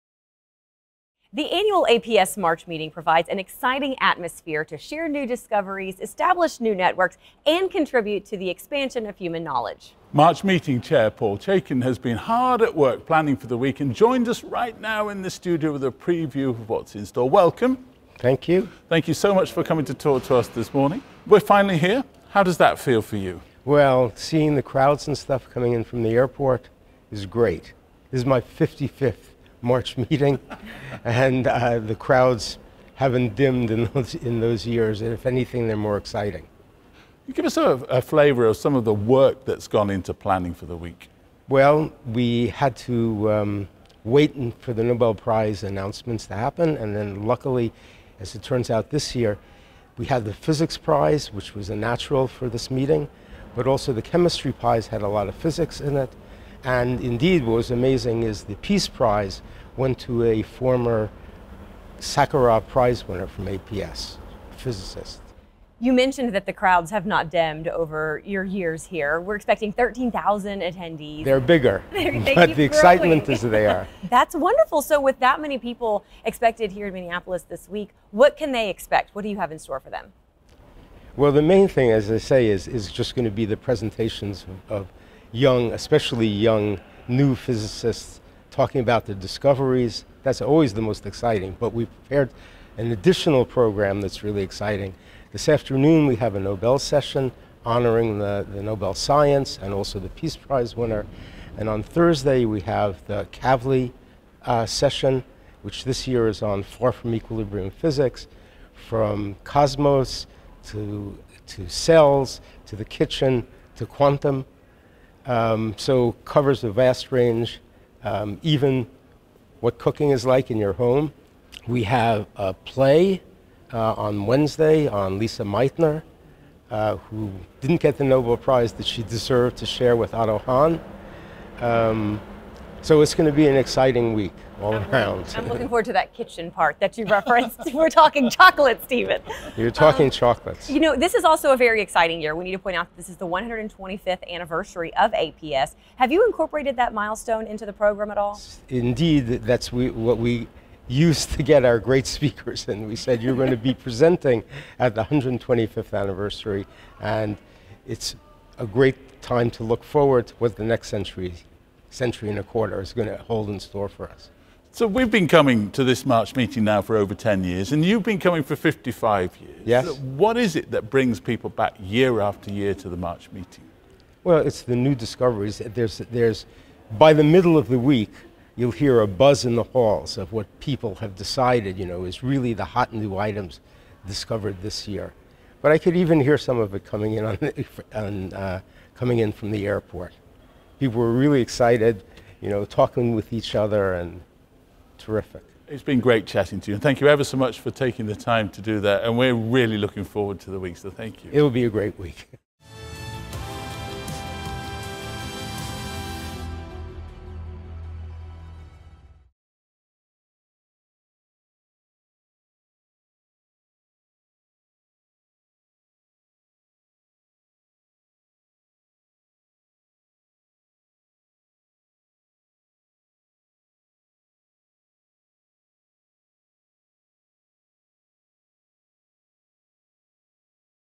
Welcome to the March Meeting! Meeting Chair Paul Chaikin joins APS TV
The annual APS March Meeting provides an exciting atmosphere to share new discoveries, establish new networks, and contribute to the expansion of human knowledge.
March Meeting chair Paul Chaikin has been hard at work planning for this week and joins us in the APS TV studio with a preview of what’s in store this week.